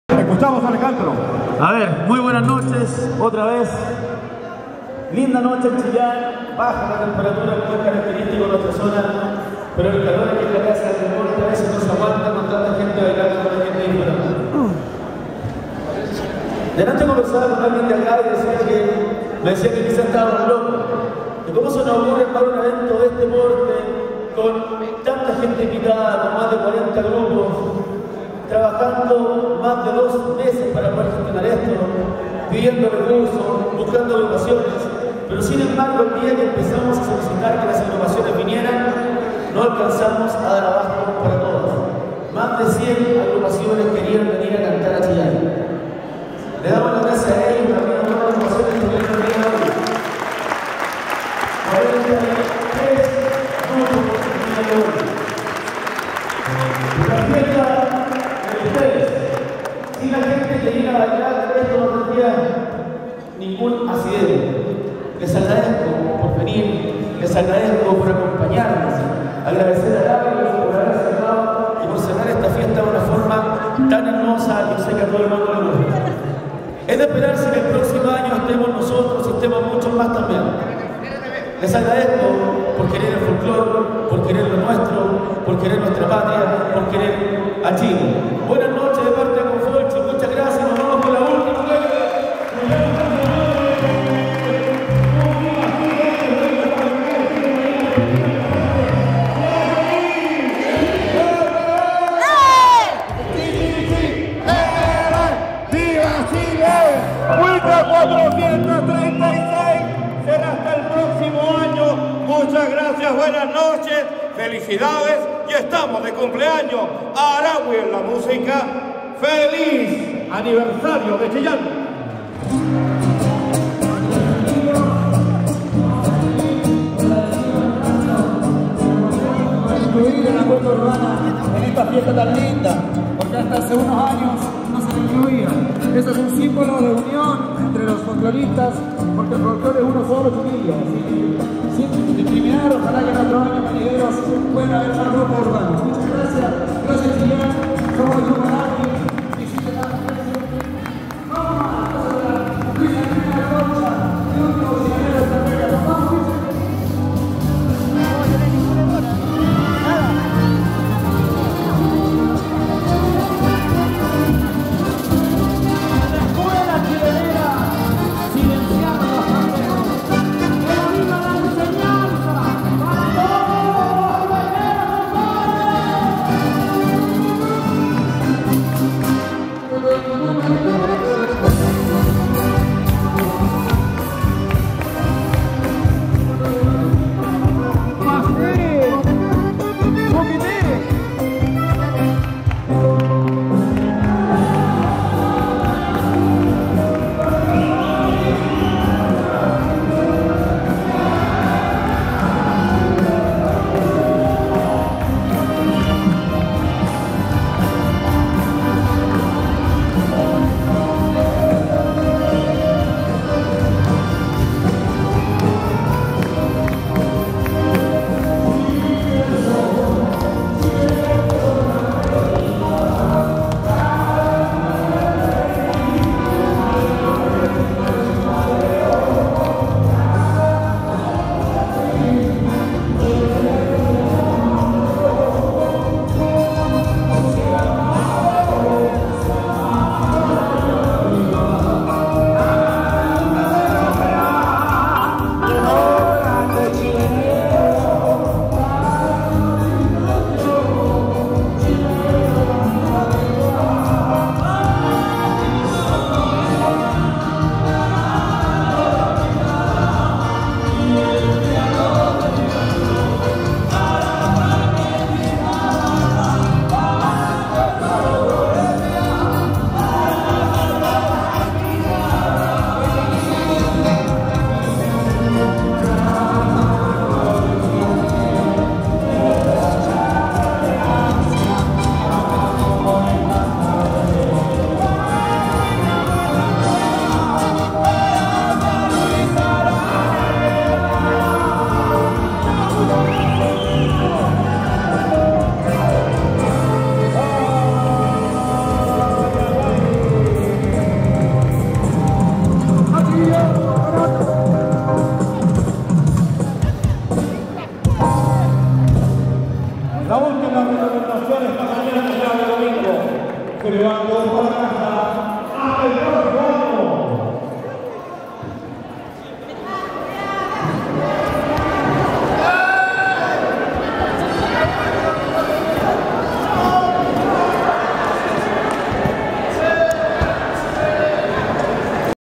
Jornada 436 Cuecas Aniversario 2016 de Chillán - Vídeo 50 Y FINAL
25 de junio 2016. VÍDEO FINAL - PALABRAS DE LOS ORGANIZADORES CON GRAN EMOCIÓN, GRATITUD Y SATISFACCIÓN AL MOMENTO DE BAILARSE LA CUECA N° 436 DE ESTA EXTENSA PERO SIGNIFICATIVA OCASIÓN. .- Casa del Deporte. Asistieron grupos folclóricos y parejas locales y de distintas ciudades del país como Los Ángeles, Antofagasta, Concepción, Valdivia, Curicó, Temuco, Copiapó, San Carlos, entre otros. La cueca 436 se bailò a eso de las 01:25 hrs. de la madrugada del domingo 26 de junio, día en que Chillán cumple 436 años de su primera fundación. ¡Felicidades Intercomuna Chillán-Chillán Viejo, tierra de Bernardo O'Higgins! Fotos en Facebook Intercomuna Chillán-Chillán Viejo. Créditos: Luis Gavilán Rojas.